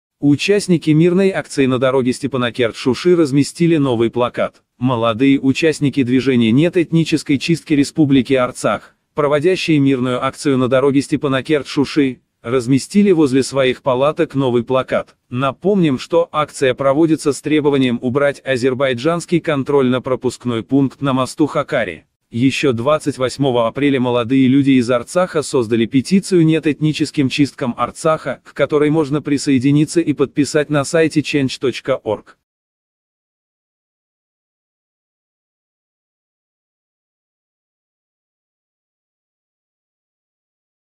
Участники мирной акции на дороге Степанакерт Шуши разместили новый плакат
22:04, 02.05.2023
Регион:Карабах
Тема: Политика, Общество

Молодые участники  движения «Нет этнической чистке Республики Арцах», проводящие мирную акцию на дороге Степанакерт-Шуши, разместили возле своих палаток новый плакат.
Напомним, что акция проводится с требованием убрать азербайджанский контрольно-пропускной пункт на мосту Хакари.
Еще 28 апреля молодые люди из Арцаха создали петицию «Нет этническим чисткам Арцаха», к которой можно присоединиться и подписать на сайте change.org.

Армения
Арцах
Карабах
Азербайджан
Срочные новости

Экстренные новости
Новости дня
Վերջին լուրեր
Յութուբ լուրեր
Շտապ